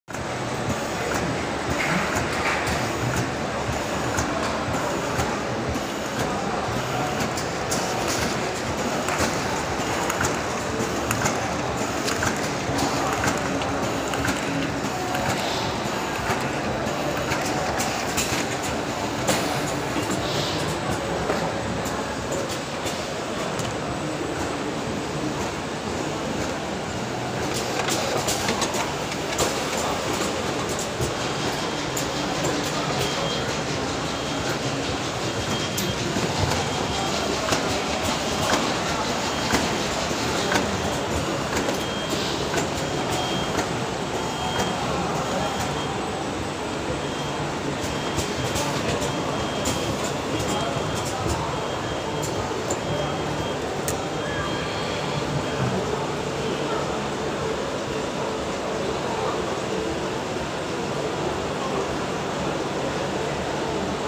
10pcs cigarettes box cellophane wrapping packing machine
This machine can wrap 10pcs box at same time
Widely used for cigarettes box,Condiment box,Playing cards box,pill box and so on.

Zhengzhou Cambrian Machinery And Electronic Co.,Ltd
Skype/wechat/line: susiemachinery